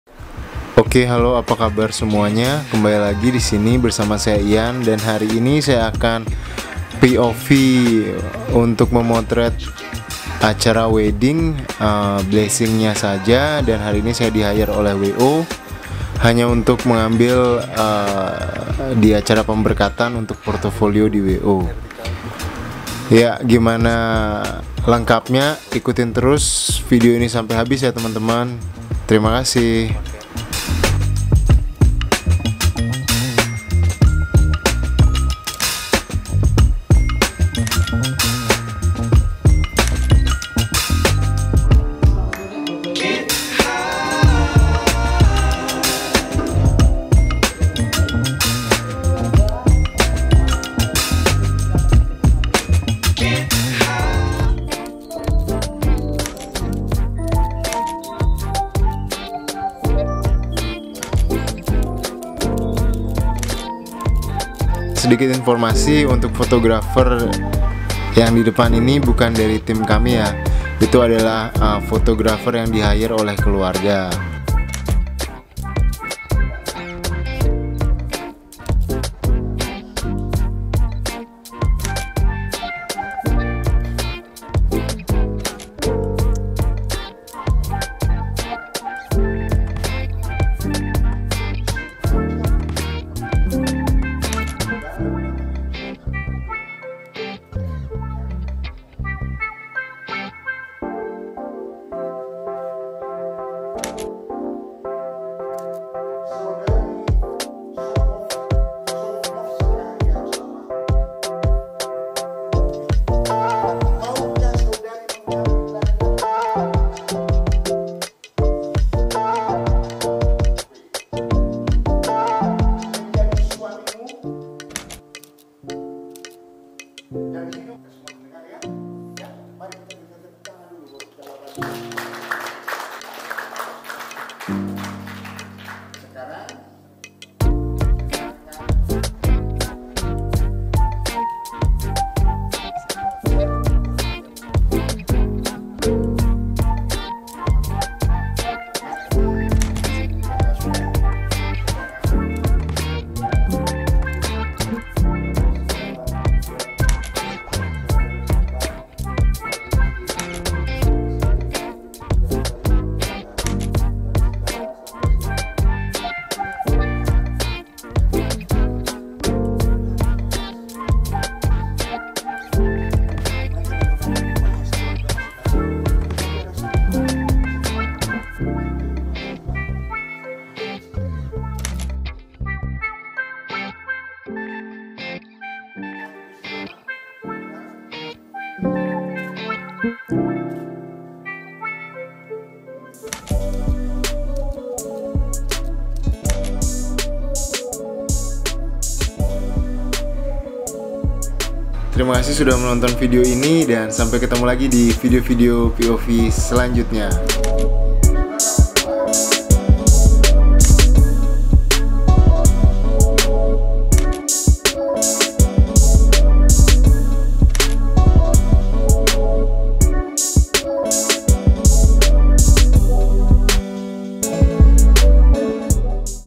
POV Wedding F&Y | Cano EOS RP
POV Wedding Eps. 06

Ian Septian adalah seorang Wedding  photographer yang tinggal di bali. dan kali ini sedang meliput salah satu wedding yang dikerjakan beberapa minggu lalu. Selamat menonton & sampai jumpa di Video Selanjutnya.